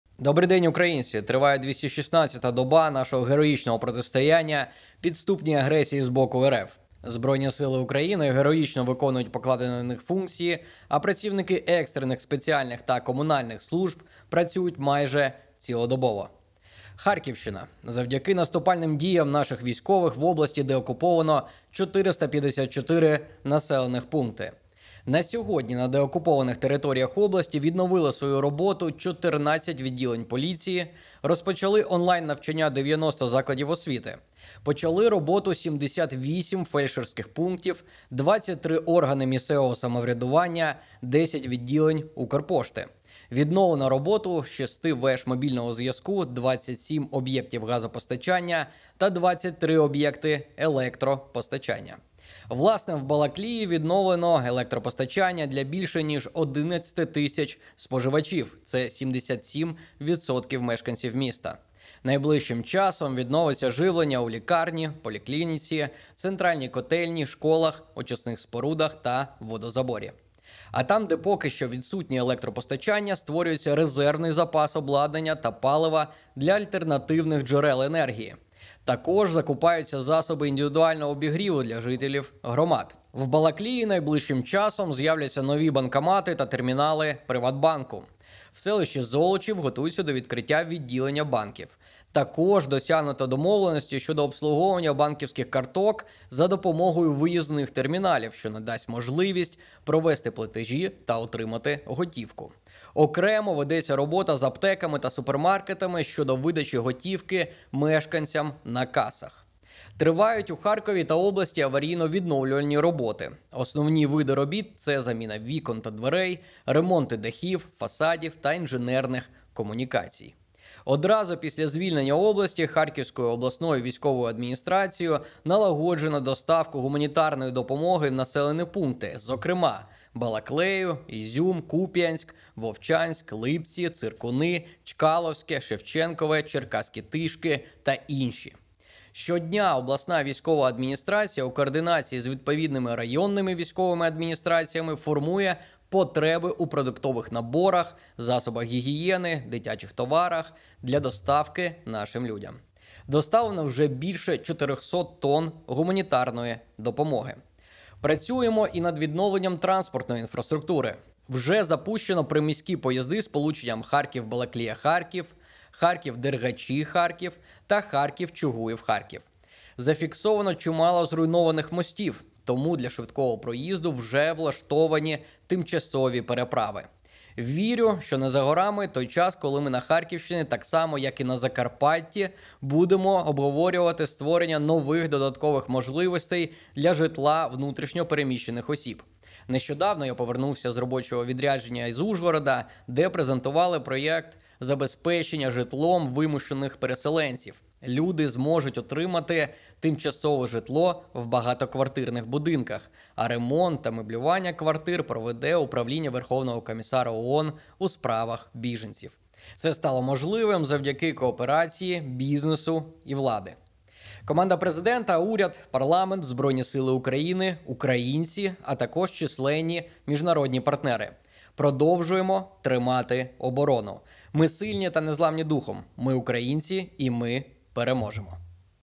Брифінг заступника керівника Офісу Президента Кирила Тимошенка (27.09.2022)
Брифінг заступника керівника Офісу Президента України Кирила Тимошенка щодо відновлення інфраструктурних обʼєктів та електропостачання на Харківщині.